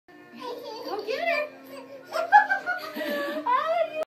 Brayden Walks!!
First steps caught on camera! So proud of our little guy!